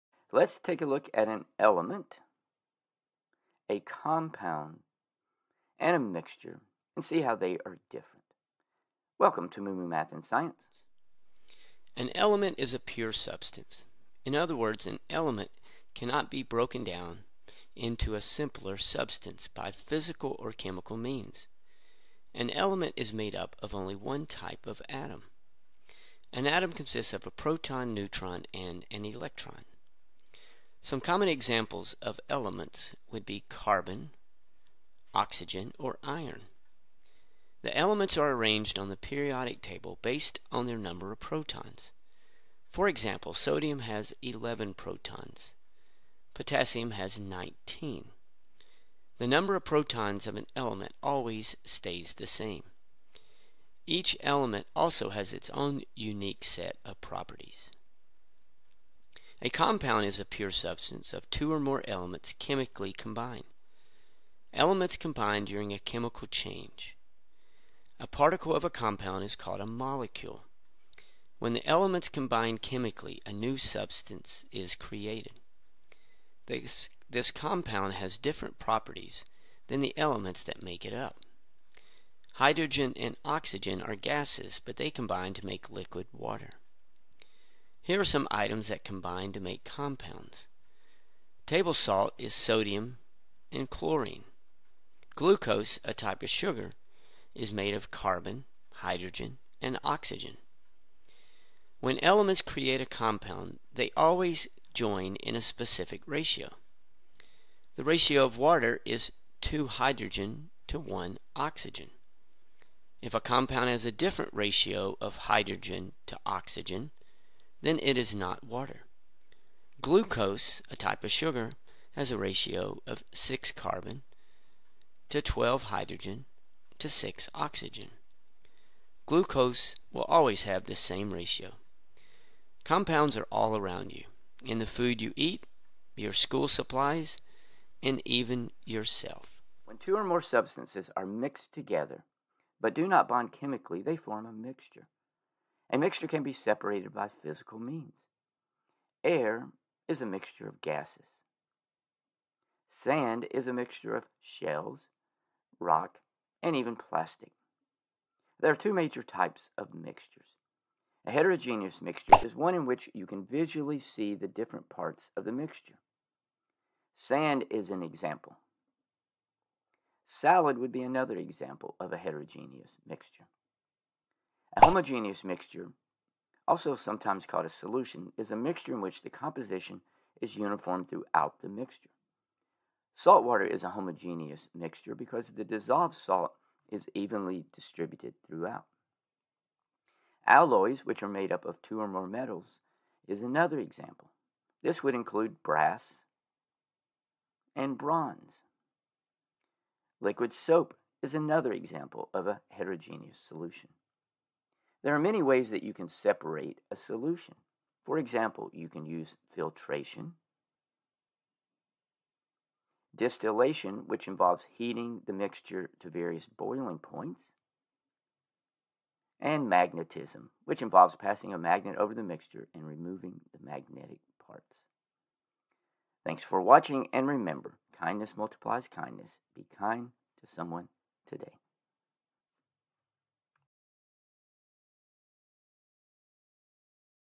Difference between an Element,Compound, and a Mixture
Learn the difference between an element, a compound, and a mixture. An element has the same number of protons, a mixture is two or more elements chemically combined, and a mixture is not chemically combined.

Aprende la diferencia entre un elemento, un compuesto y una mezcla.

English
This video has been dubbed into Spanish (United States) using an artificial voice to increase accessibility. You can change the audio track language in the Settings menu.